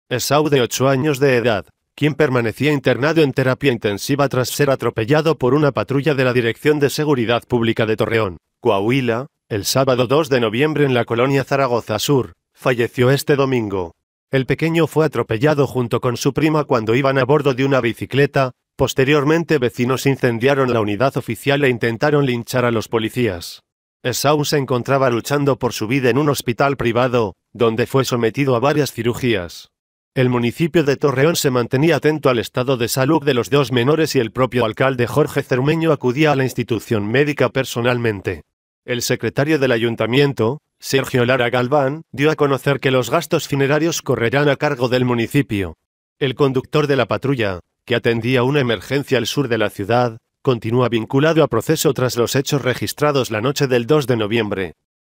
Muere niño que fue atropellado por patrulla en Coahuila
Esaú de 8 años de edad, quién permanecía internado en terapia intensiva tras ser atropellado por una patrulla de la Dirección de Seguridad Pública de Torreón, Coahuila, el sábado 2 de noviembre en la colonia Zaragoza Sur, falleció este domingo.

El pequeño fue atropellado junto con su prima cuando iban a bordo de una bicicleta, posteriormente vecinos incendiaron la unidad oficial e intentaron linchar a los policías.

Esaú se encontraba luchando por su vida en un hospital privado, donde fue sometido a varias cirugías.

El municipio de Torreón se mantenía atento al estado de salud de los dos menores y el propio alcalde Jorge Zermeño acudía a la institución médica personalmente.

El secretario del Ayuntamiento, Sergio Lara Galván, dio a conocer que los gastos funerarios correrán a cargo del municipio.

El conductor de la patrulla, que atendía una emergencia al sur de la ciudad, continúa vinculado a proceso tras los hechos registrados la noche del 2 de noviembre.